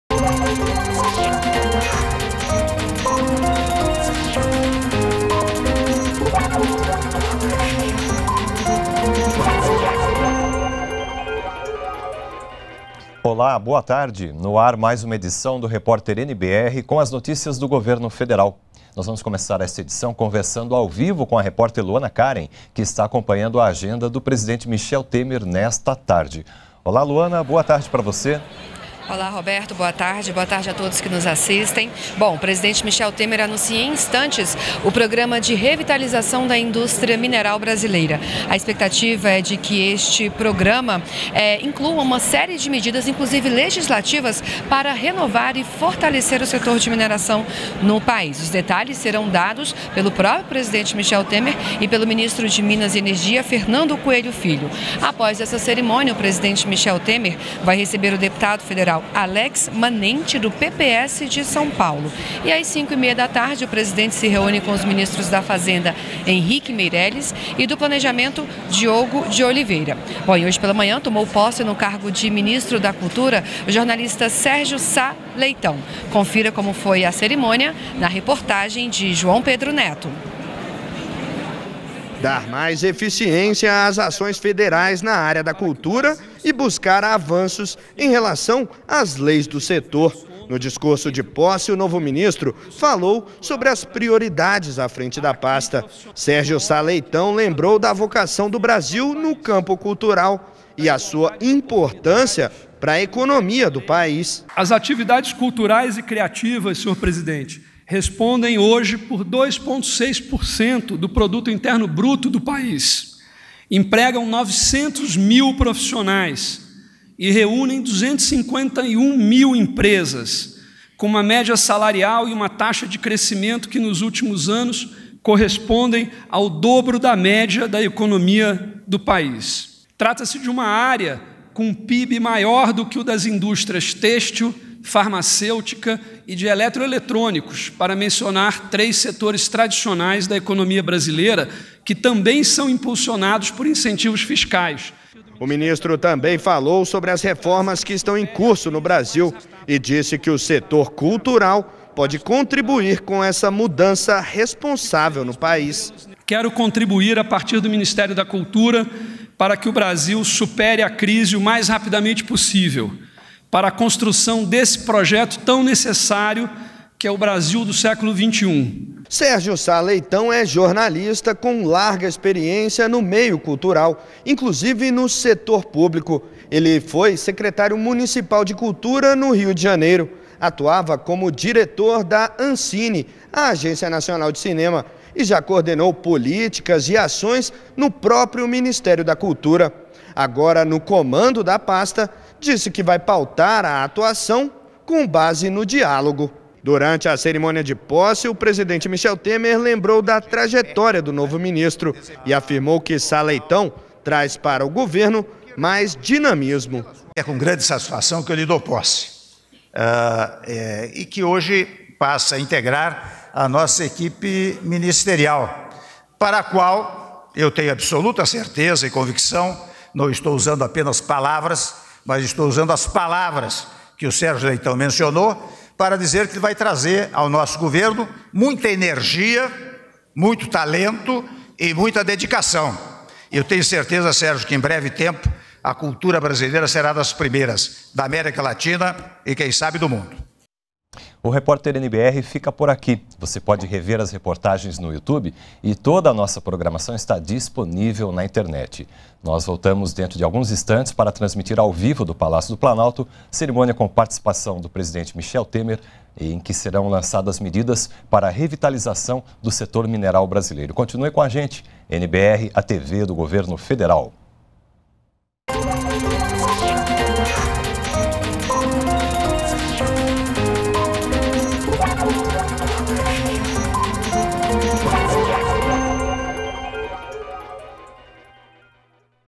SNJ lança edital para desenvolver ações de combate à violência contra os jovens
REPÓRTER NBR 16H - 25.07.17: A Secretaria Nacional de Juventude lançou um edital para que estados e municípios possam captar recursos para desenvolver ações como o combate à violência contra os jovens. Os estados e os municípios devem enviar as propostas de adesão ao programa entre os dias 4 de agosto e 2 de setembro. Começaram hoje as inscrições para o FIES. O prazo vai até até sexta-feira, dia 28.